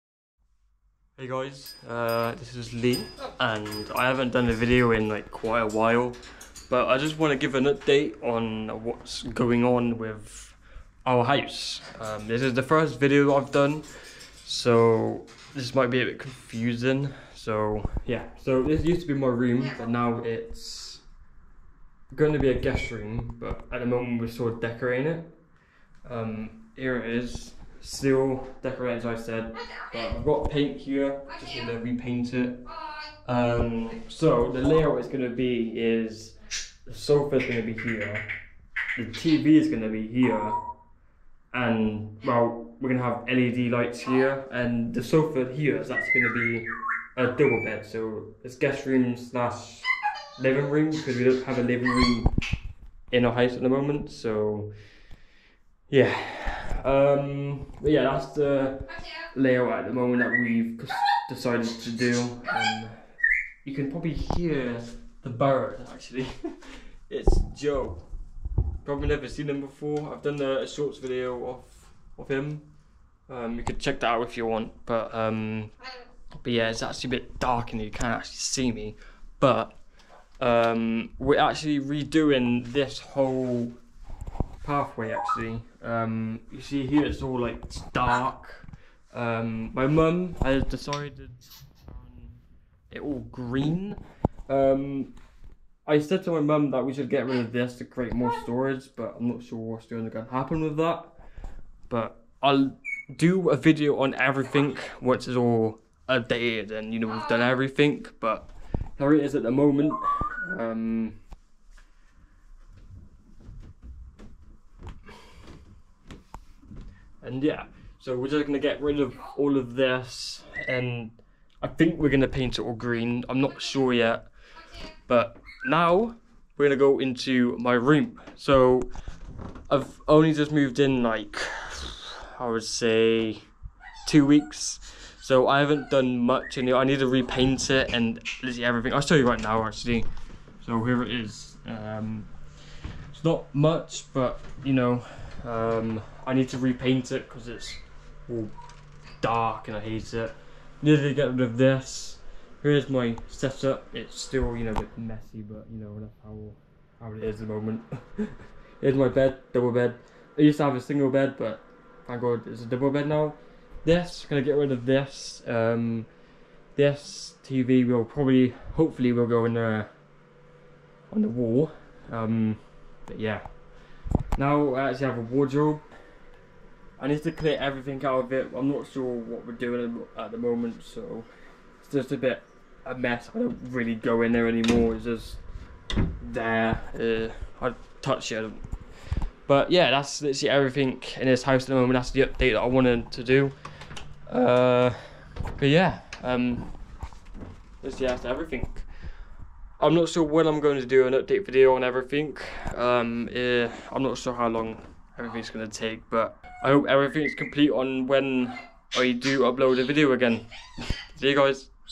Update on My Room & Working On Guest Room - 1 September 2024
In this video, I'll give you an update on my room and share the process of decorating .